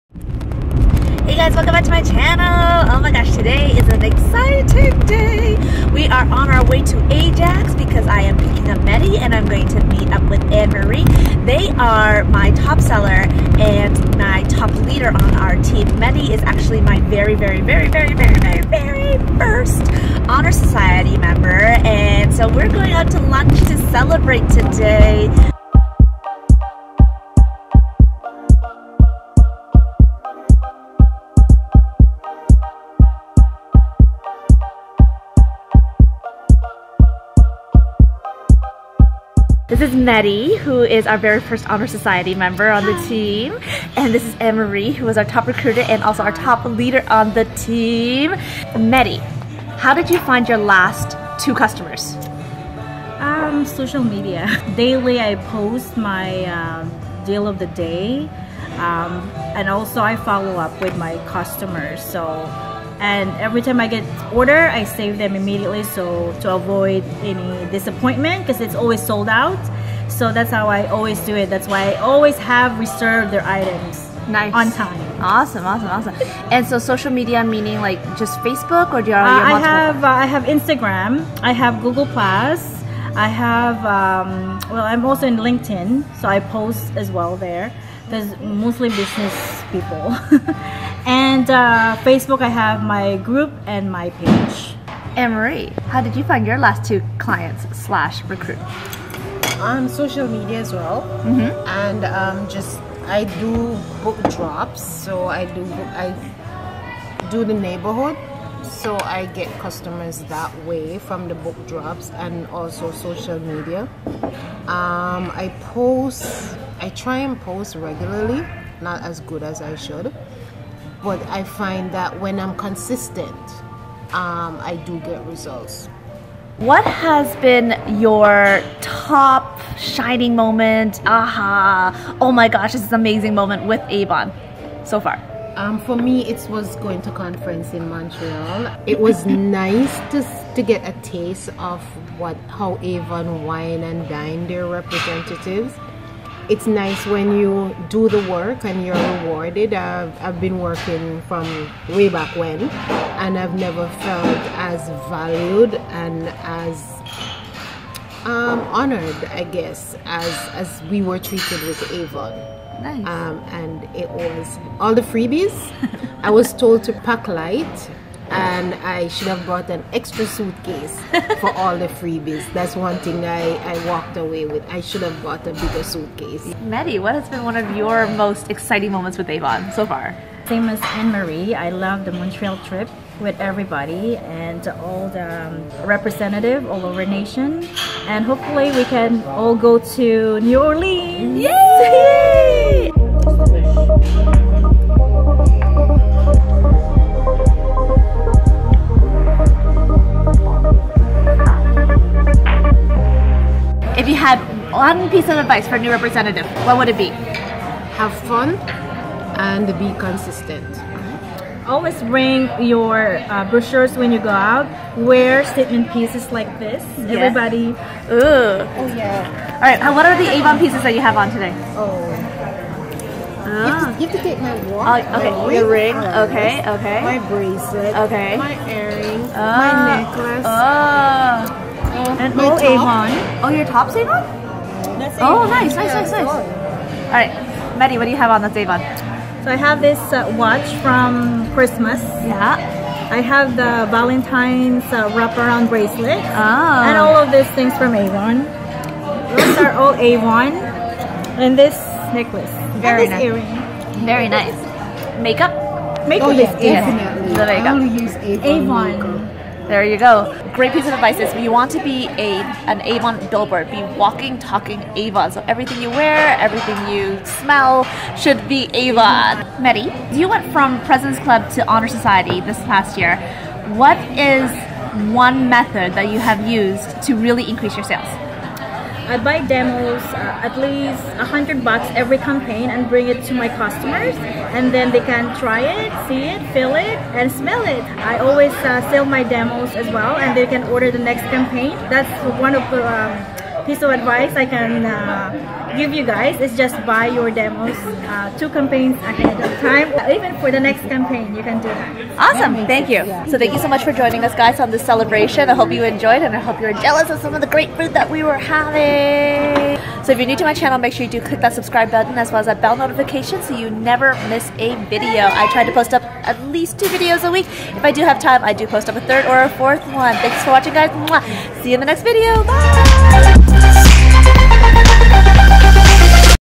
Celebrating and Learning from the Top! | Lunch Celebration + Business tips!
Join our Lunch Celebration with my top seller and top leader for 2018!  They share some great business tips on how they have been growing their business plus you get to check some of the yummy food we got to enjoy.

xo's

Jessie